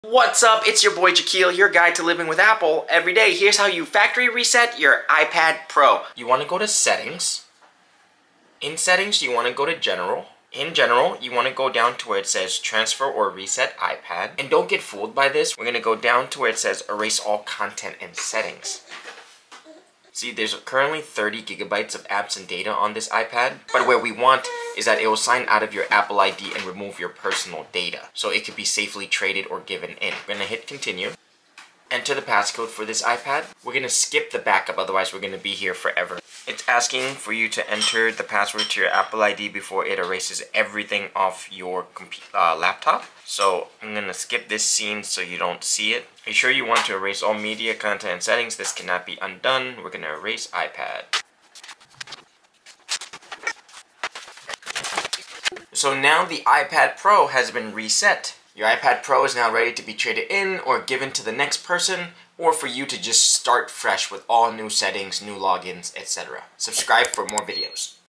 How To Factory Reset Your iPad Pro In Less Than A Minute!
In this video, I show you how to reset your iPad Pro to factory settings in less than a minute!